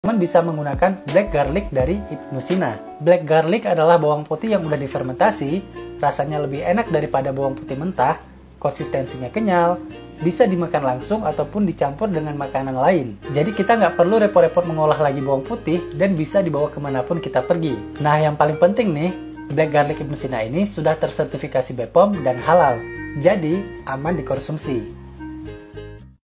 Review Black Garlic Ibnu Sina | dr. Ary Andri
Black Garlic Ibnu Sina merupakan bawang putih tunggal yang telah difermentasi dalam suhu dan jangka waktu tertentu, sehingga bawang yang tadinya berwarna putih berubah menjadi hitam dengan antioksidan yang jauh lebih tinggi dibandingkan bawang putih biasa.

Bawang hitam ini mengandung senyawa aktif seperti SAC, polifenol dan hidrogen sulifida yang mampu membantu menurunkan darah tinggi secara alami.

Black Garlic Ibnu Sina telah mendapat banyak review bagus dari pelanggan maupun dokter, seperti dr. Ary Andri

Yuk tonton review Black Garlic Ibnu Sina dari dr. Ary Andri ini sampai akhir ✨

Facebook : Black Garlic Ibnu Sina